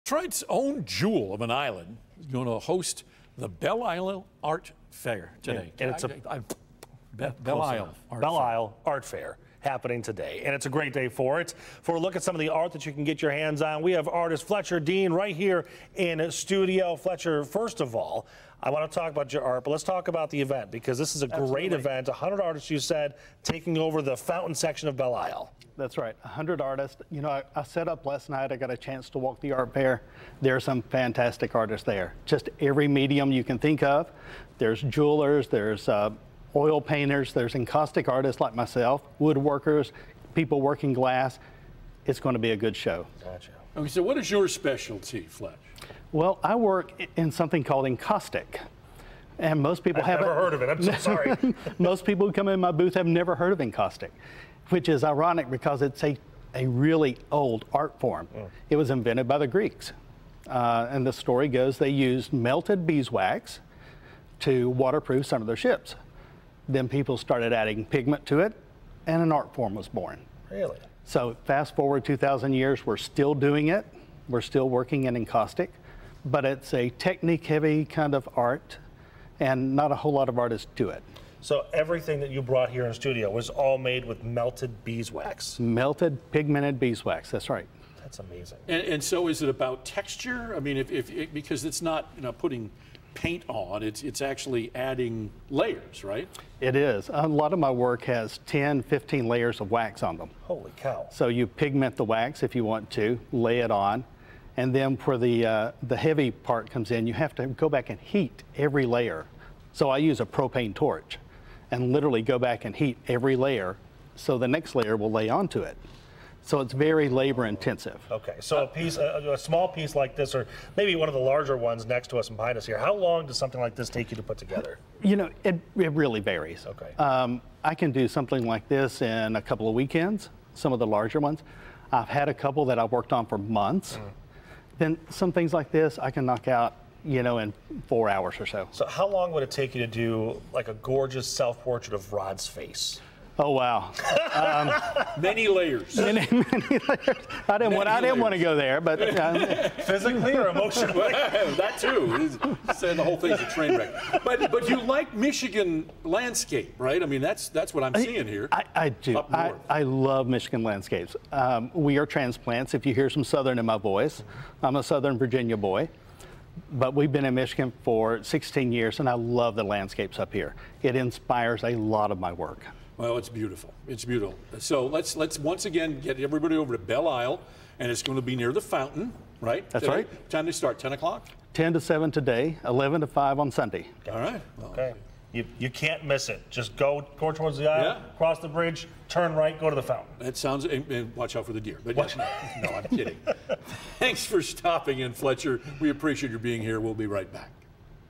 What to know about the Belle Isle Art Fair
The Belle Isle Art Fair is an art show across from the Scott Fountain in the heart of Detroit's island park.